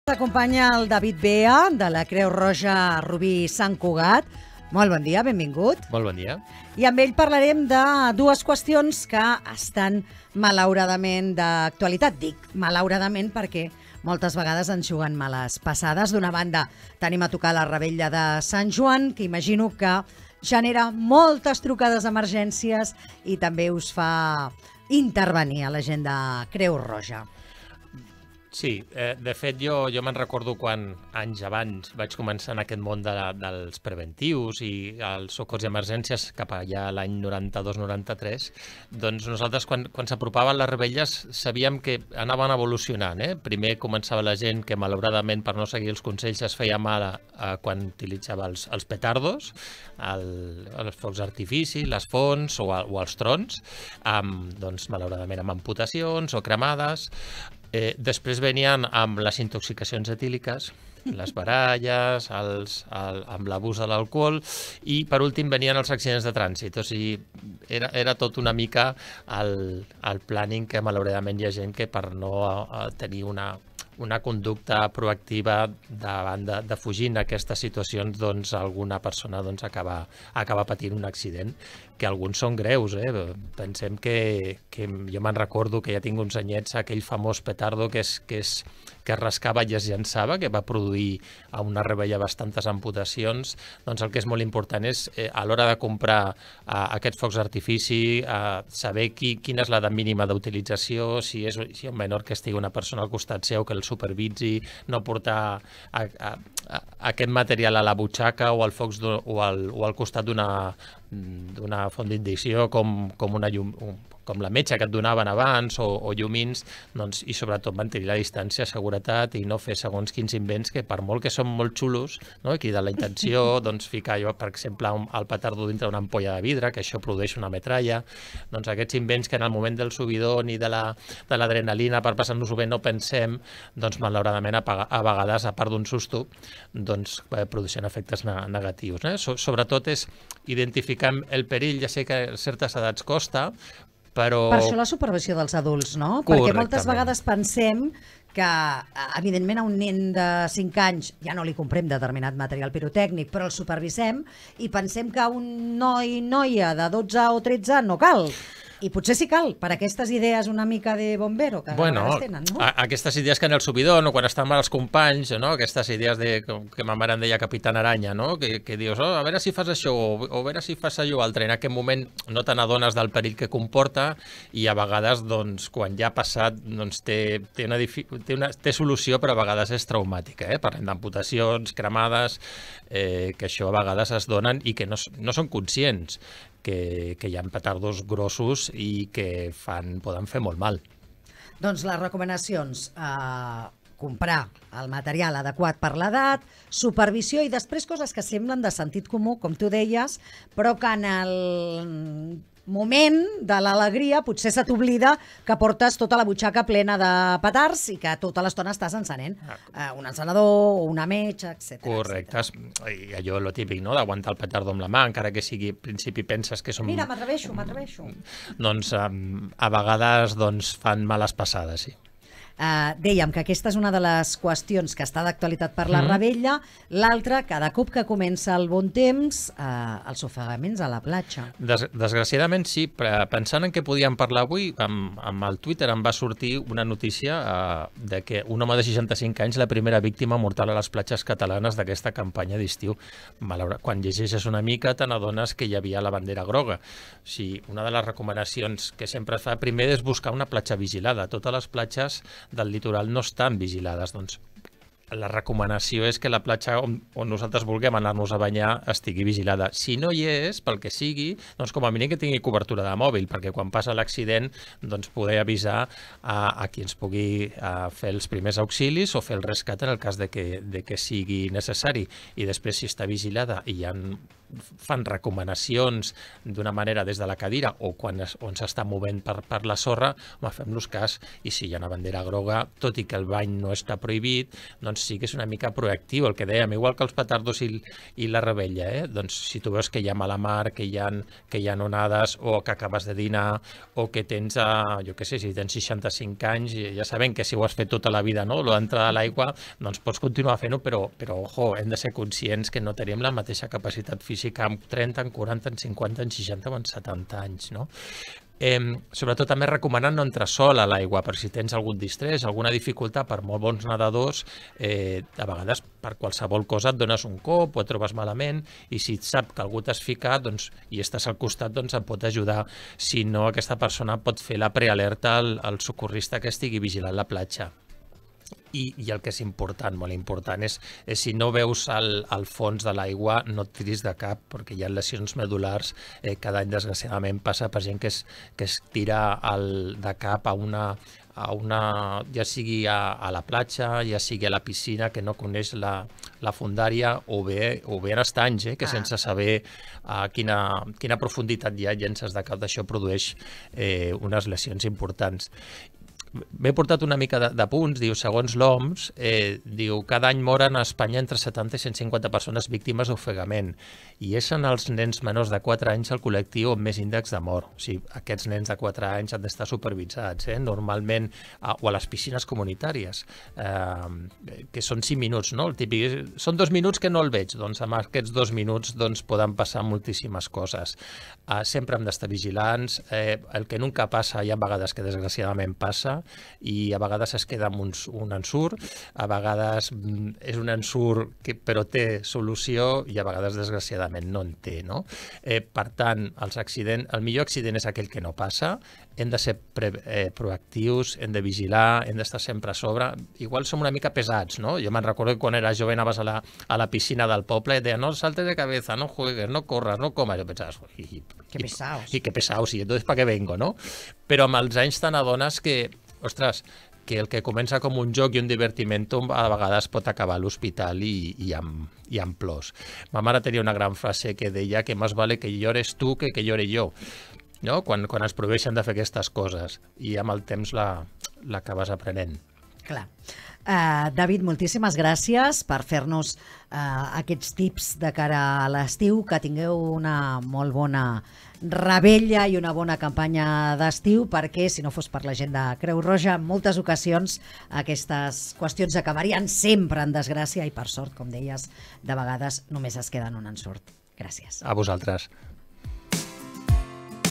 Seguretat a les platges i revetlles
David Bea de la Creu Roja Rubí-Sant Cugat ens dona consells de seguretat per tenir revetlles segures. A més, a l'inici de la temporada de bany, Bea recorda que cal tenir precaució a l'hora de banyar-se al mar.